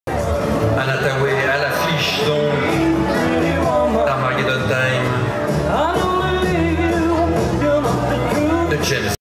戛纳2022：茱莉亚·罗伯茨现身《世界末日》戛纳红毯 招牌笑容太有感染力
茱莉亚·罗伯茨身着一件颇有中性风格的西装连体裤。出现在红毯的罗伯茨，打扮得清新自然，招牌笑容颇有感染力。
据悉亮相戛纳的罗伯茨，并没有主演的电影要上映，她其实是投资方邀请来的嘉宾。不过罗伯茨现在也不轻松，依然有工作要忙，她的新电影《离开这个世界》（Leave The World Behind）正在纽约拍摄中。